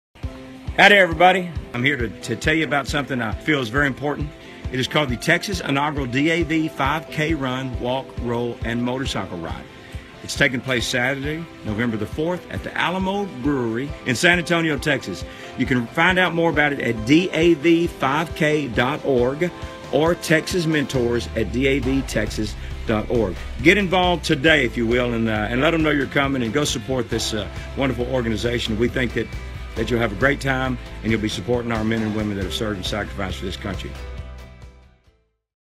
PSA McCoy San Antonio DAV5K
Country music superstar Neal McCoy invites you to run, walk, roll, or ride a motorcycle for the DAV 5K in San Antonio this year, November 4th.